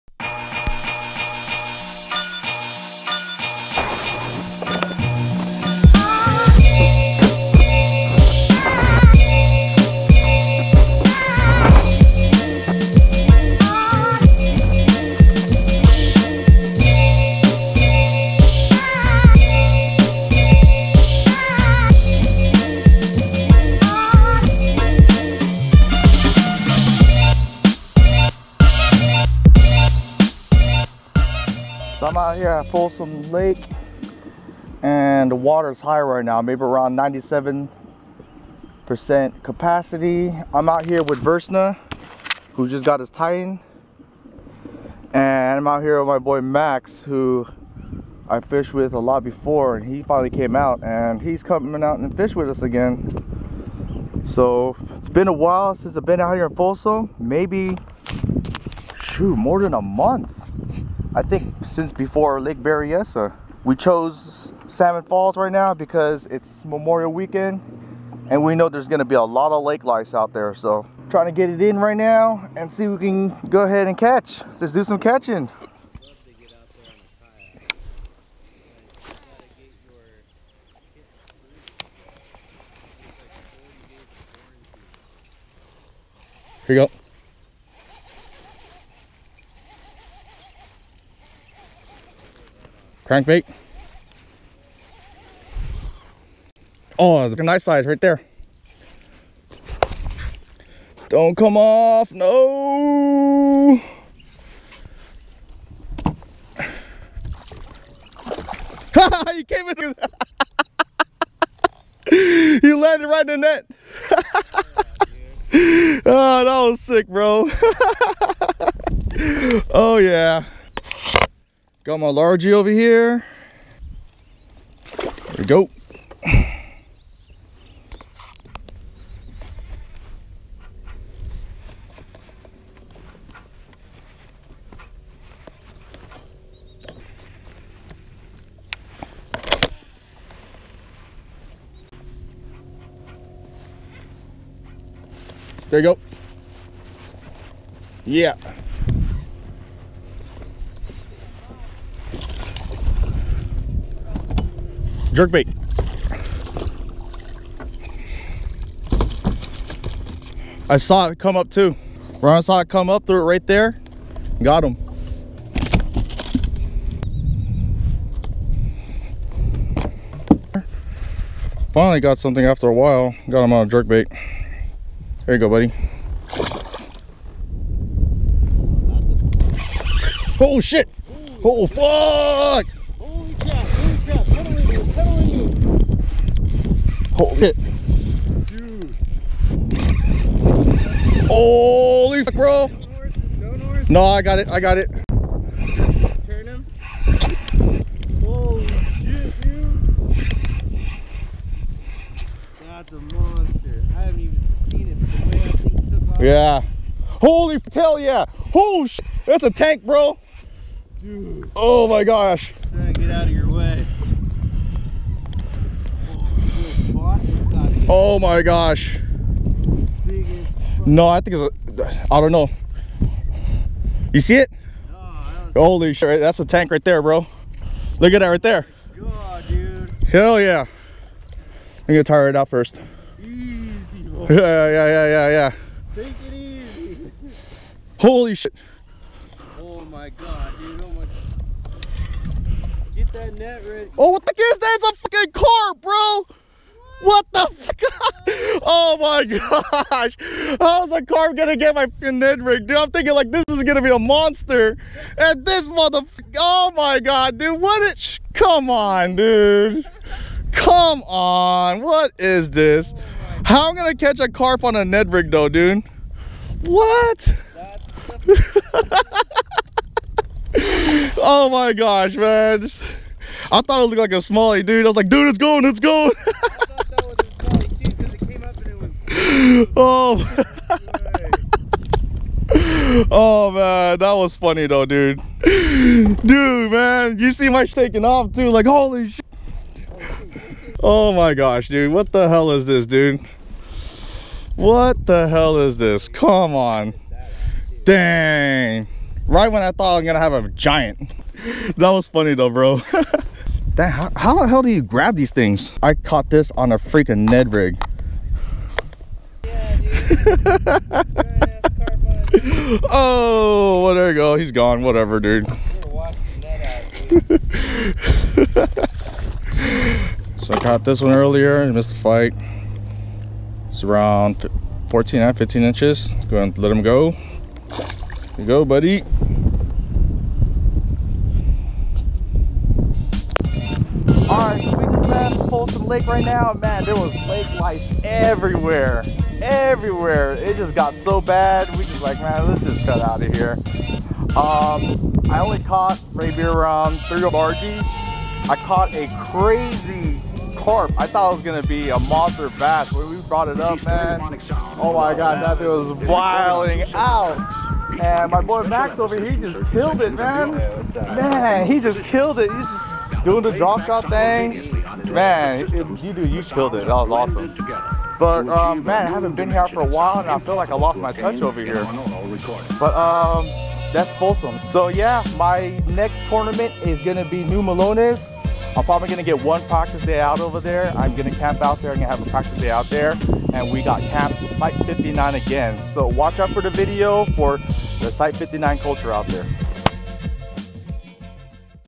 Fishing Salmon Falls at Folsom Lake
Short video of a morning session at Salmon Falls in Folsom Lake. Its Memorial Day Weekend and wanted to get a quick session in before all the Lake Lice (jetskis and weekend boaters) roll in.